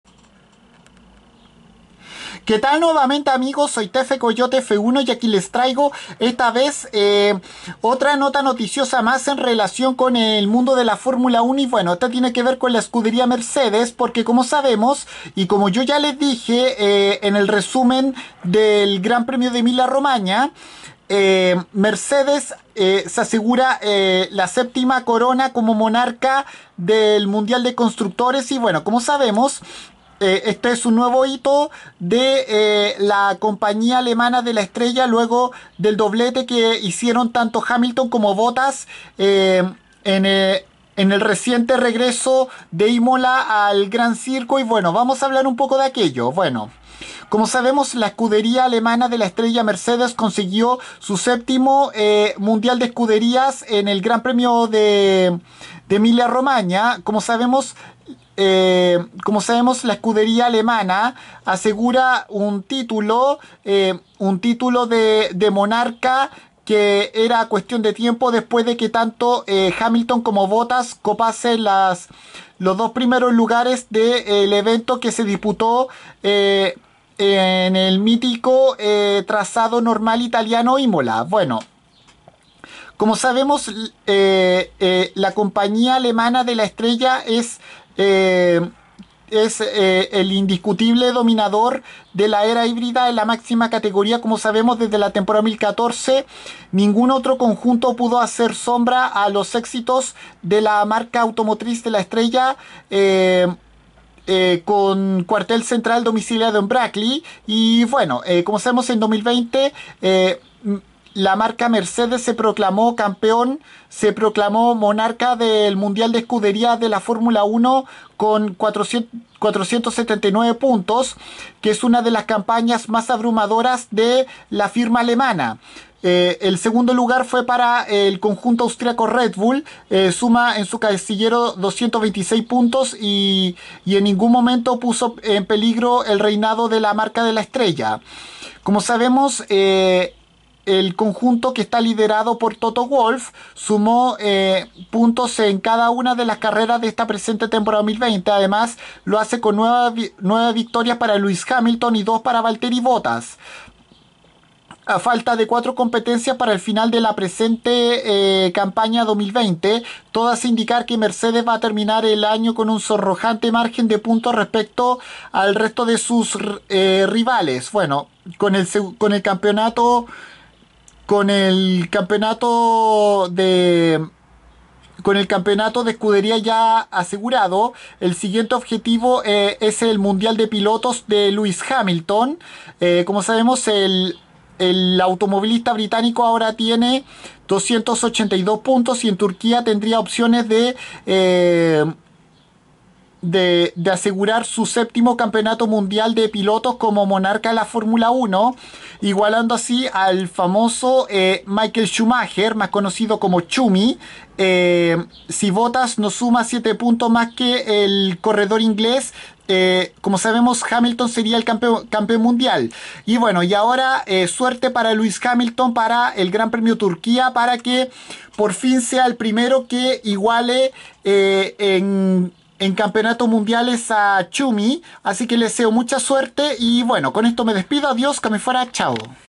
Mercedes AMG Petronas Formula One Team se proclama monarca de escuderías 2020 en la Fórmula 1
Por 7ma vez consecutiva la escudería alemana de la estrella con cuartel central domiciliado en Brackley gana el campeonato de marcas de la Temporada 2020 de Fórmula 1 en la reciente versión 2020 del Gran Premio de Emilia Romaña que trajo de vuelta al Gran Circo al Autódromo Enzo e Dino Ferrari de la ciudad italiana de Imola

Además la automotriz alemana supera a Scuderia Ferrari como el constructor que ganó mas veces de manera consecutiva en la Fórmula 1